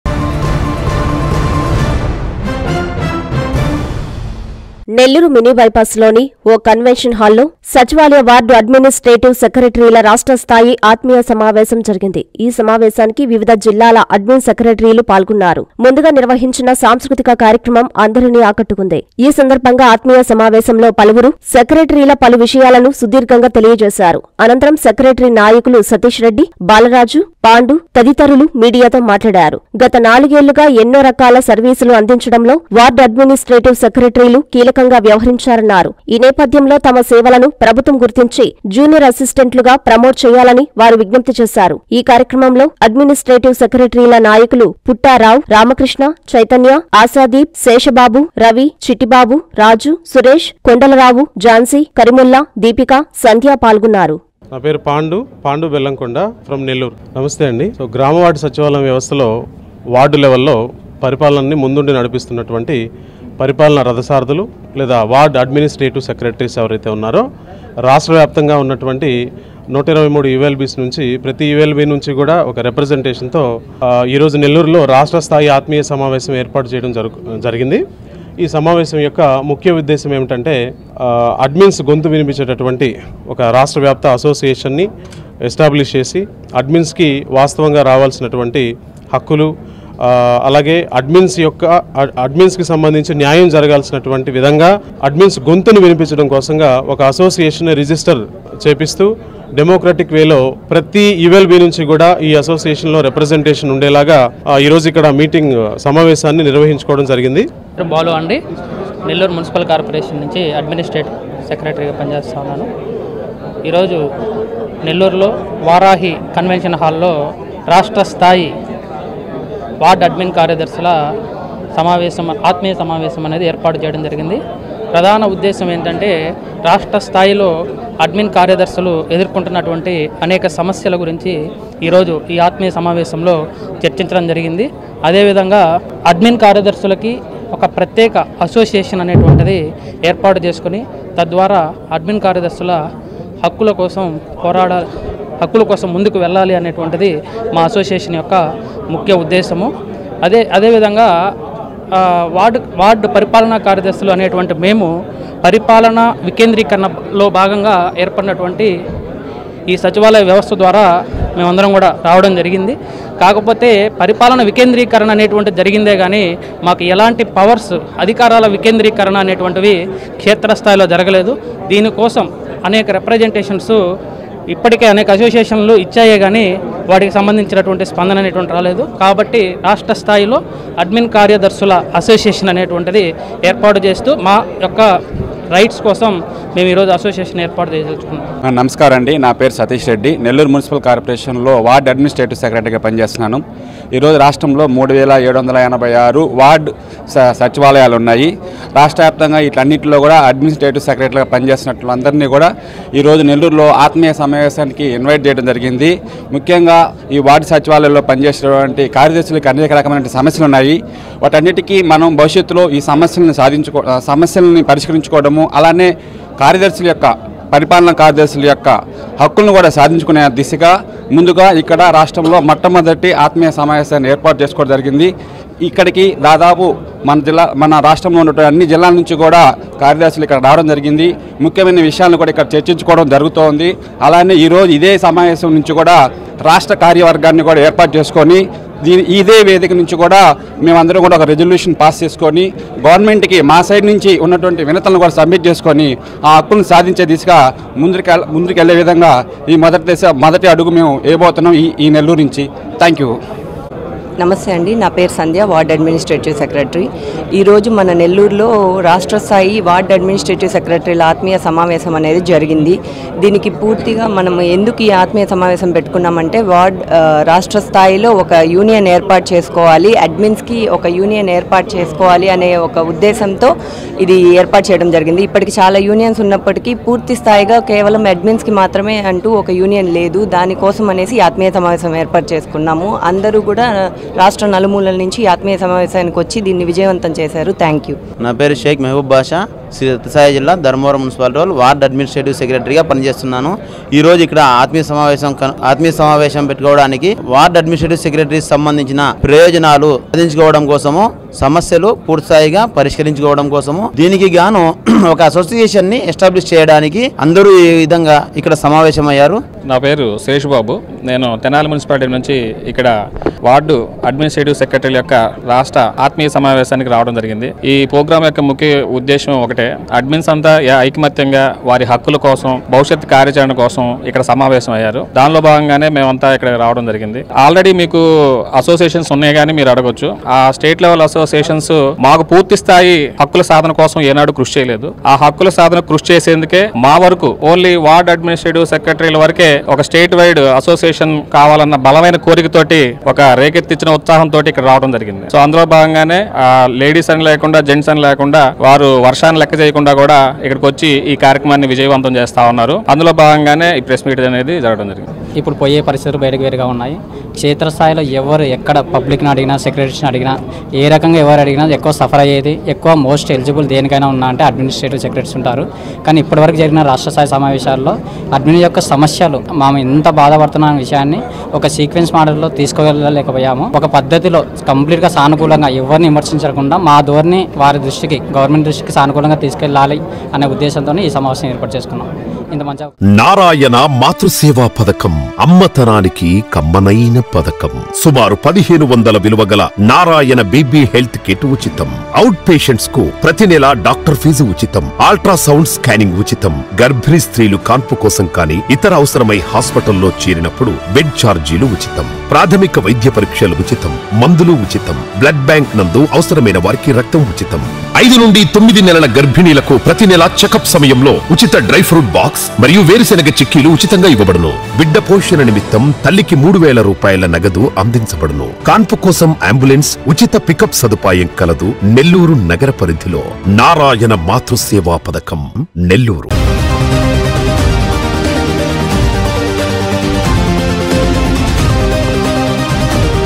జూనియ‌ర్ అసిస్టెంట్లుగా ప్ర‌మోట్ చేయాలి
- వార్డ్ అడ్మినిస్ట్రేటివ్ సెక్ర‌ట‌రీల సేవ‌ల‌ను ప్ర‌భుత్వం గుర్తించాలి
- గ‌త నాలుగేళ్లుగా ప్ర‌జ‌ల‌కి ఎన్నో ర‌కాలు స‌ర్వీస్‌లు అందించాం
- వార్డ్ అడ్మినిస్ట్రేటివ్ సెక్ర‌ట‌రీలు
- నెల్లూరులో రాష్ట్ర స్థాయిస్థాయి ఆత్మీయ స‌మావేశం